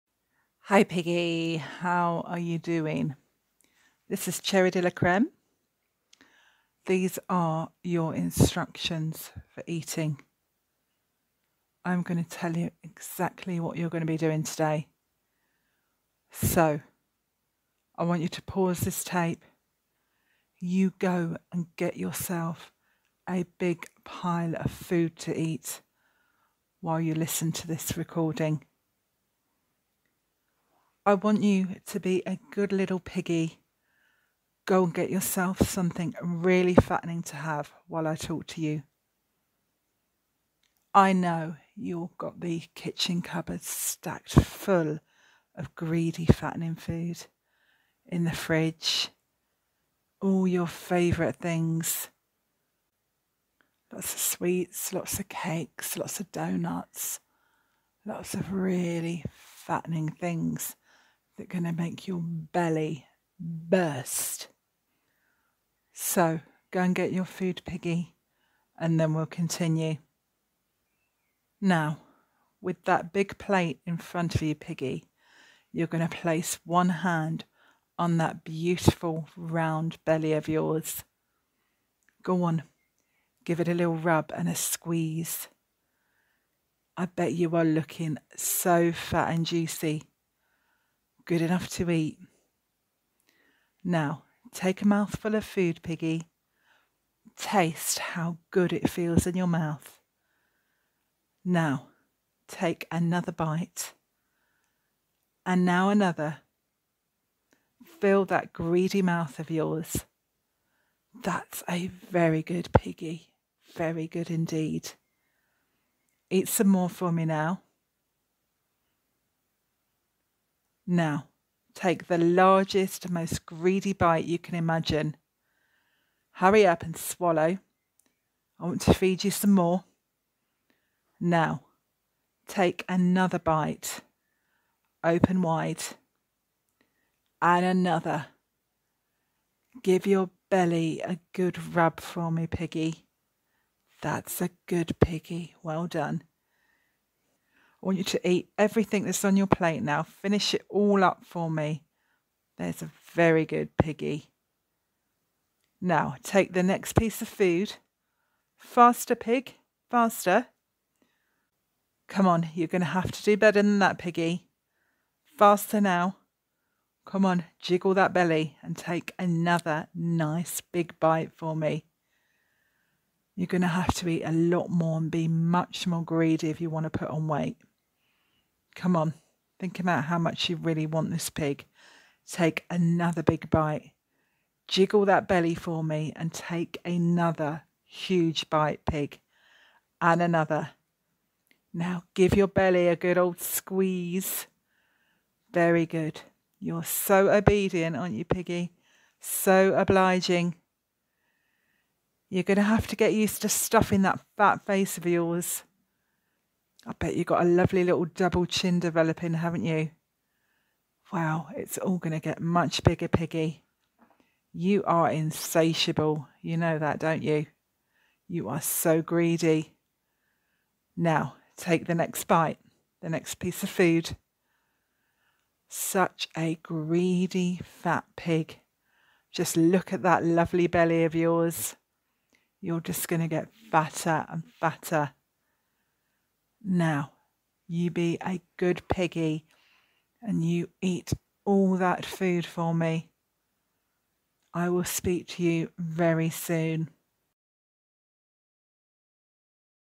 Instructions for eating (feederism and gaining weight encouragement for greedy pigs)
I've had a couple of requests for this video and I'd like you to listen whilst you gorge yourself on all your favourite fattening foods. Imagine I am there with you, personally feeding you. I wanna make all your dreams come true! This one is not too harsh.

Adventures in fatland - tales about feederism out now!

Eating encouragement for greedy pigs as part of a feederism lifestyle.